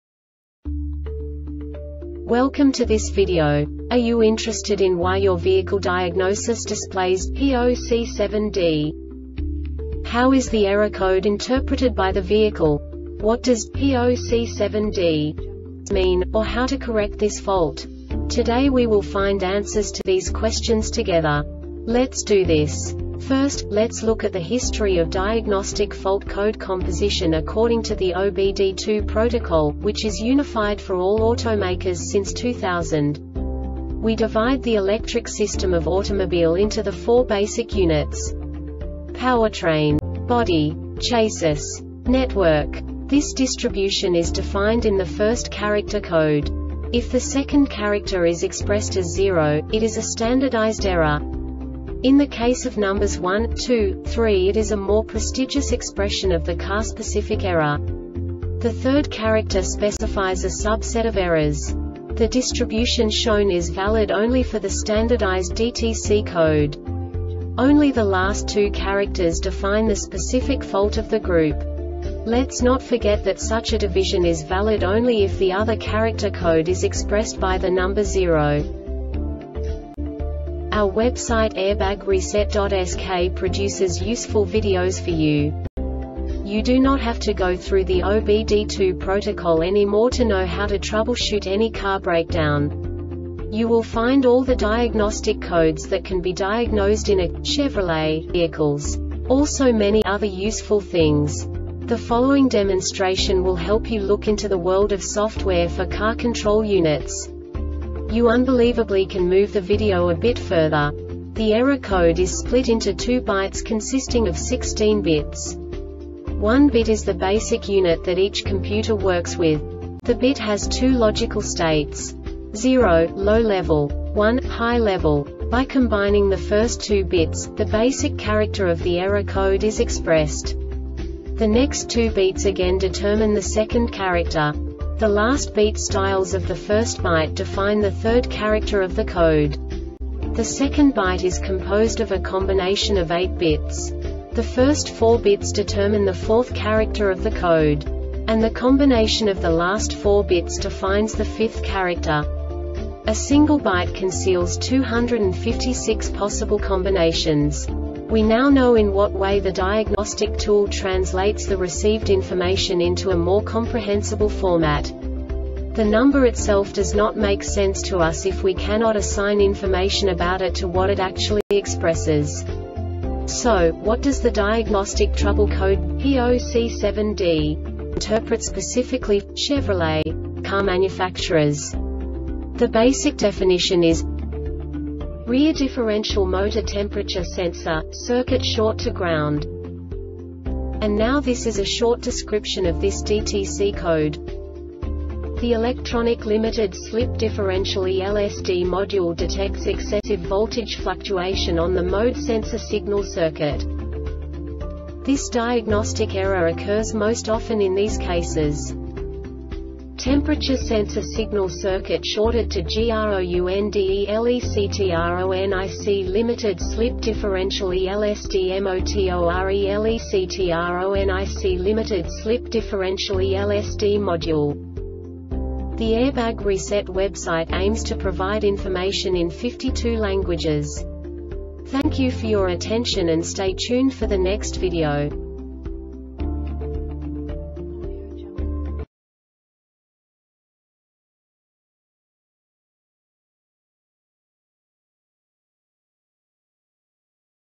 DTC Chevrolet P0C7D Short Explanation
Contents:
0:21 Basic DTC analysis according to OBD2 protocol standard.
1:48 Insight into programming
2:58 A diagnostically understandable description of the Diagnostic Trouble Code
3:05 Basic error definition

Make:
Chevrolet
Code:
P0C7D
Definition:
Rear Differential Motor Temperature Sensor - Circuit Short To Ground
Description:
The Electronic Limited Slip Differential (ELSD) Module detects excessive voltage fluctuation on the Mode Sensor Signal circuit.
Cause:
TEMPERATURE SENSOR SIGNAL CIRCUIT SHORTED TO GROUNDELECTRONIC LIMITED SLIP DIFFERENTIAL (ELSD) MOTORELECTRONIC LIMITED SLIP DIFFERENTIAL (ELSD) MODULE